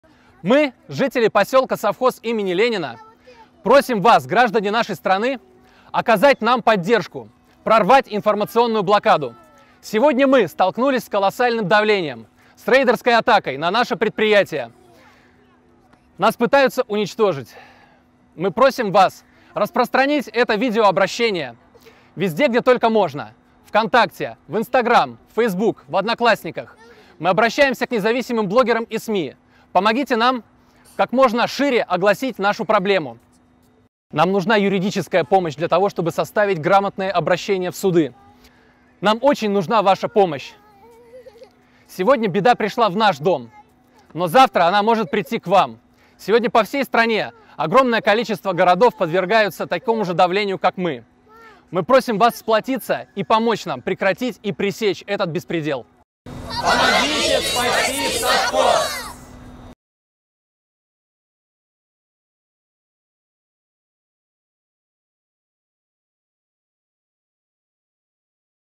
Призыв поддержать Совхоз имени Ленина был услышан, но борьба не окончена
Дорогие жители России, мы благодарим вас за поддержку. Благодаря вам мы продолжаем борьбу за маленький островок социализма в нашей захваченной олигархами стране. Мы уверены, что вместе мы обязательно отобьемся от рейдеров и начнем преобразовывать нашу Родину по примеру Совхоза имени Ленина. За Грудинина и Совхоз имени Ленина. Ура!